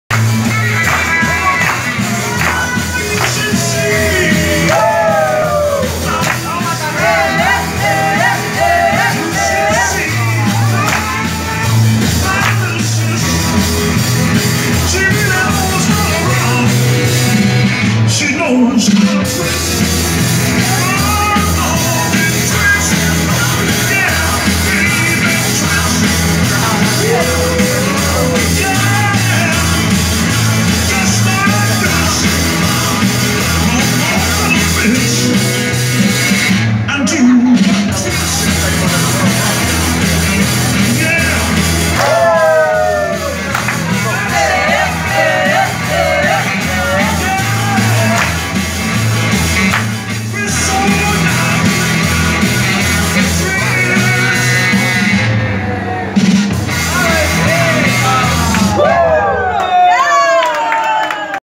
JOHNNY ROCKETS COSTANERA CENTER PROVIDENCIA (5-Agosto-2019)
EN NUESTRO VIAJE A SANTIAGO;ALMORZAMOS EN ESTE CONOCIDO LOCAL.PUDIMOS SER TESTIGOS DEL BAILE DEL PERSONAL DE ATENCIÓN DE DICHO LUGAR.POR SUPUESTO,GRABÉ DICHO MOMENTO.